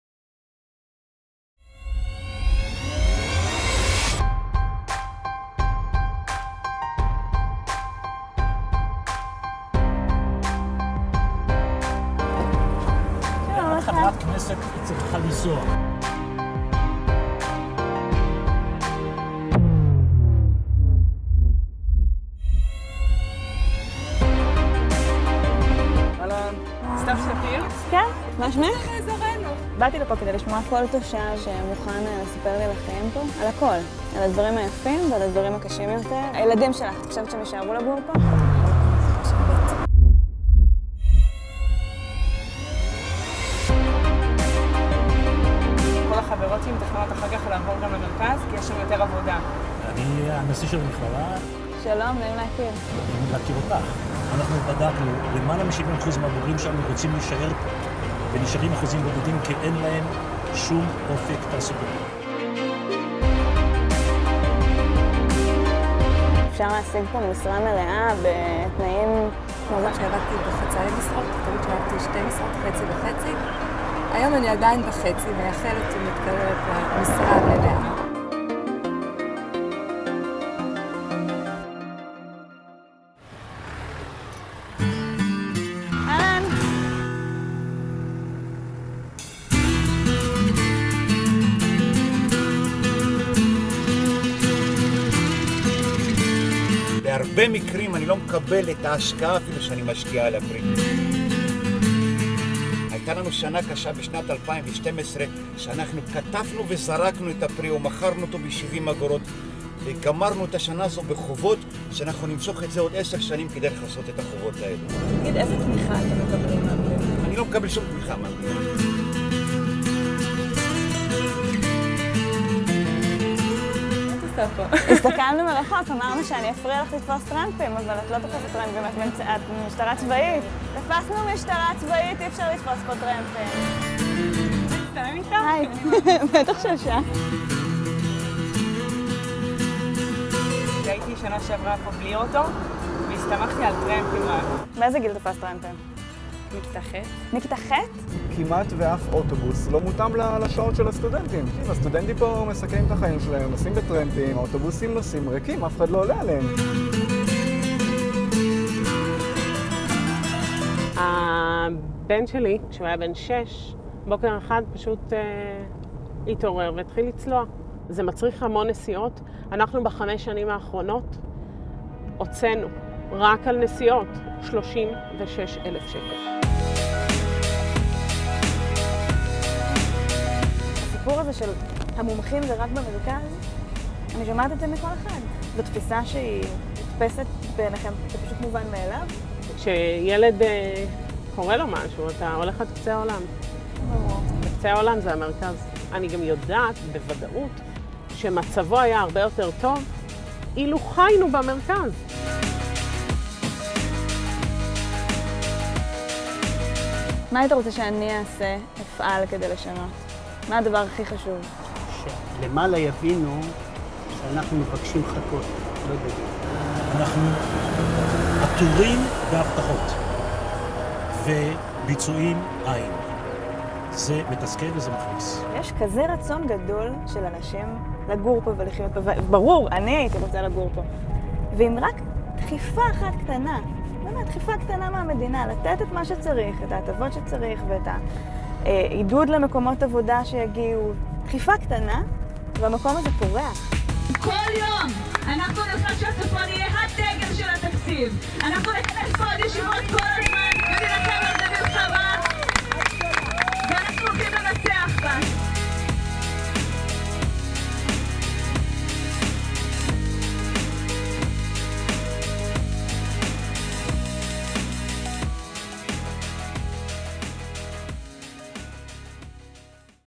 Parliament Member Stav Shafir's campaign for the budget of north Israel
שלחנו את חברת הכנסת סתיו שפיר לתפוס טרמפים